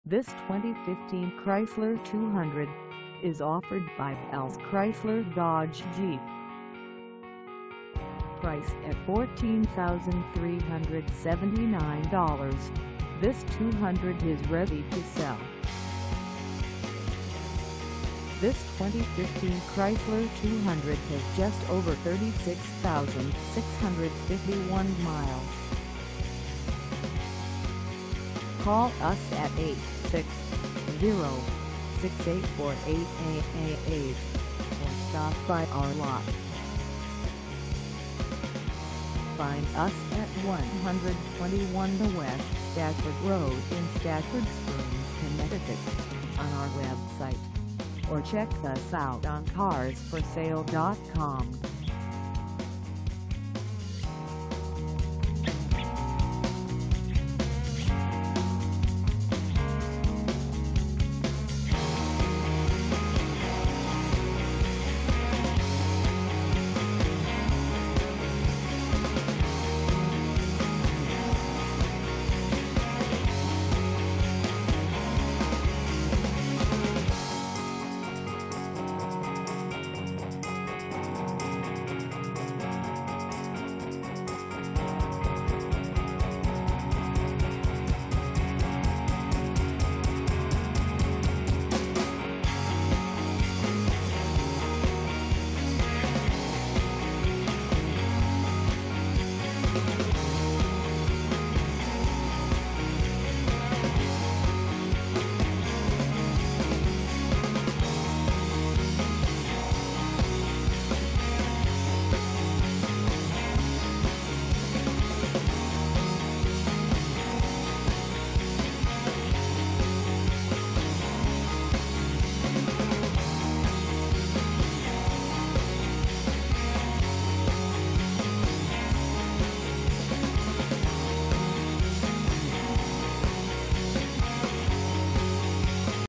2015 Chrysler 200 S 4dr Sedan for sale in Stafford Springs,
This 2015 Chrysler 200 S 4dr Sedan is for sale in Stafford Springs, CT 06076 at Bolles Chrysler Dodge Jeep.

Engine: 2.4L I4
Transmission: Automatic 9-Speed
Color: Blue
Mileage: 36651
Contact Bolles Chrysler Dodge Jeep 121 A West Stafford Rd Stafford Springs, CT 06076 to test drive this 2015 Chrysler 200 S 4dr Sedan today.